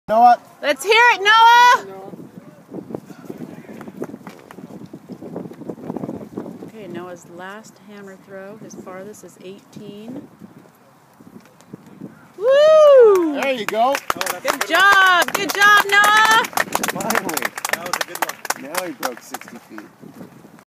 Noah Bultman Hammer throw
12 yr old Noah 2 turns short wire 4kg hammer 65-7. At Cerritos College.